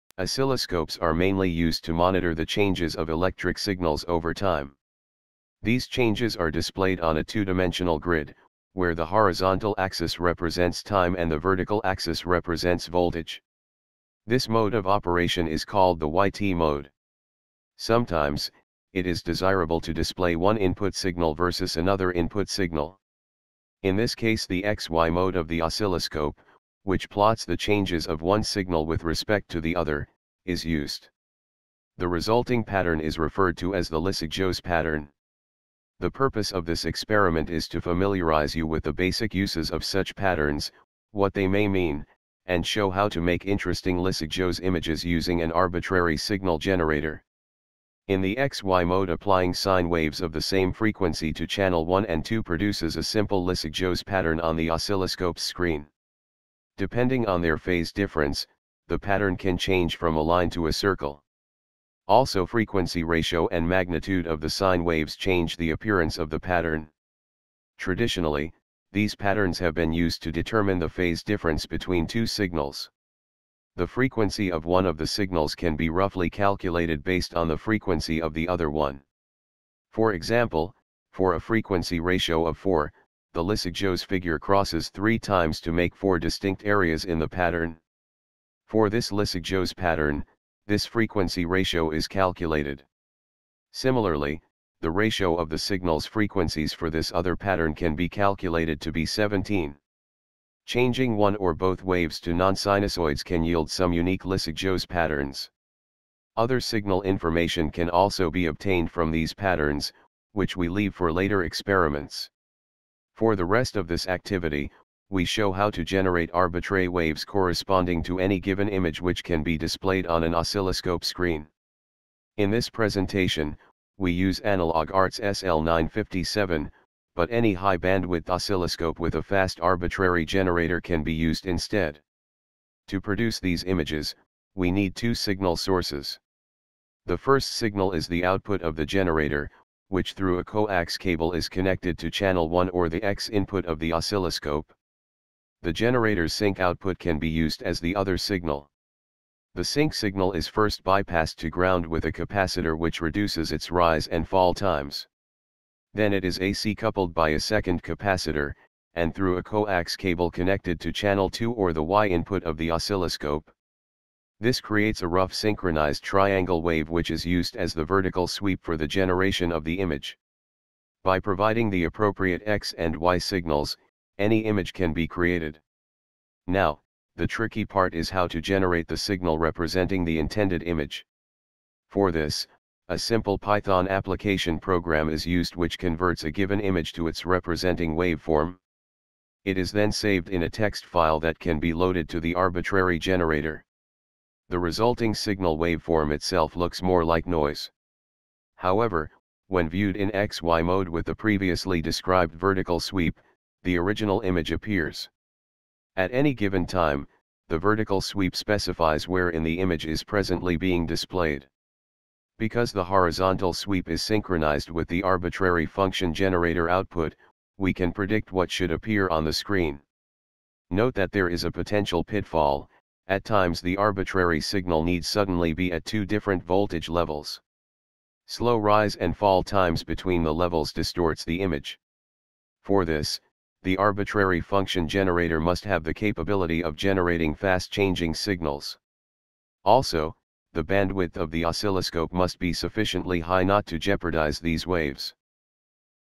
Oscilloscope X-Y Mode (Lissajous Curve)
An Analog Arts SL957 USB oscilloscope is used in this experiment to familiarize you with the basic uses of Lissajous patterns, what they may mean, and show how to make interesting Lissajous images using an arbitrary signal generator. An EDN article is a good reading material about this subject.